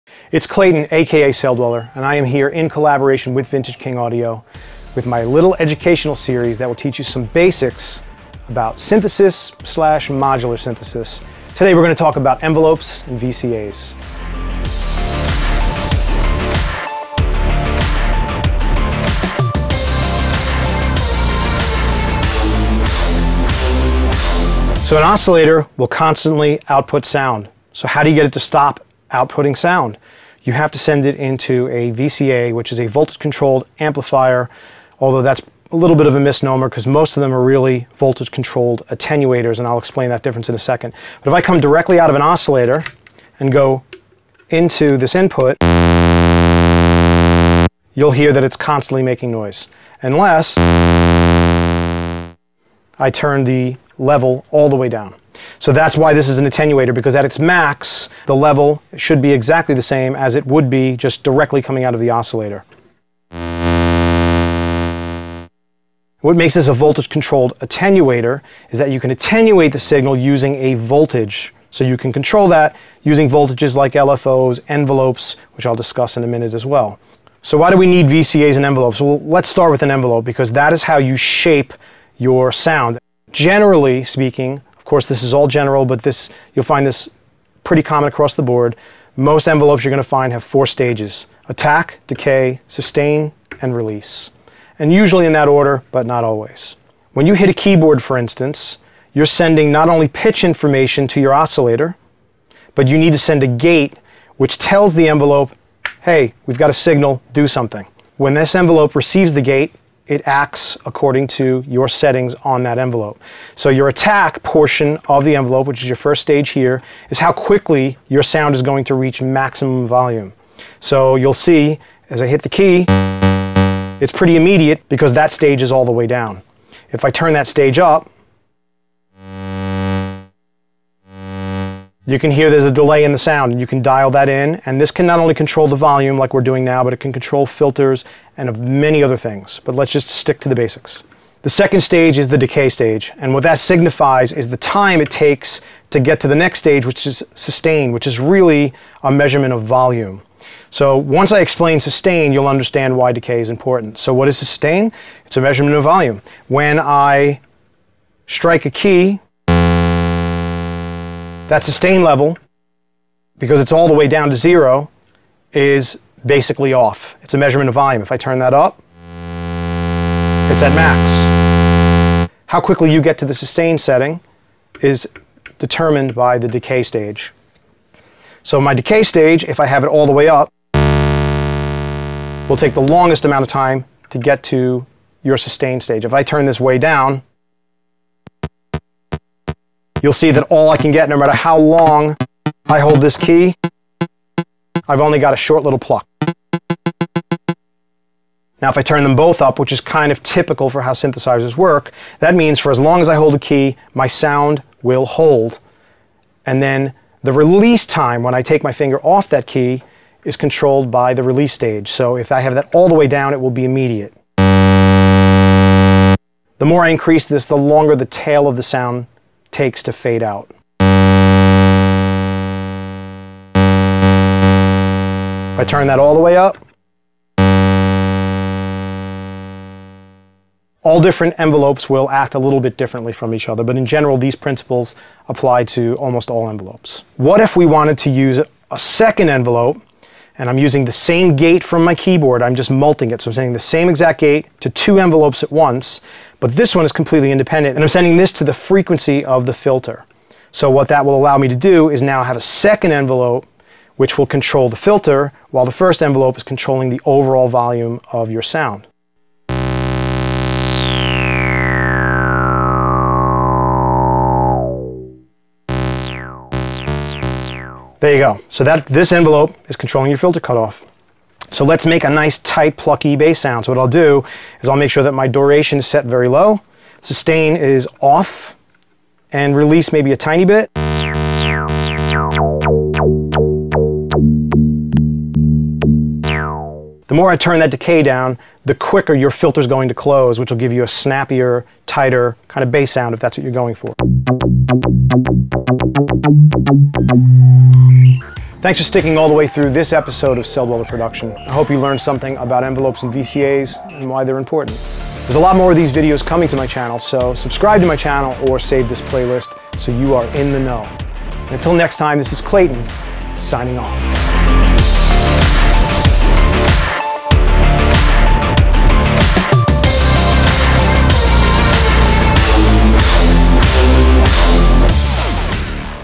Educational Series: Envelopes & VCAs
In this special educational episode of Celldweller Production, Klayton walks you through the basics of Envelopes and VCAs using a Roland System-500

Watch Klayton showcase the latest and greatest modular gear, synths and software on more episodes of Celldweller Production -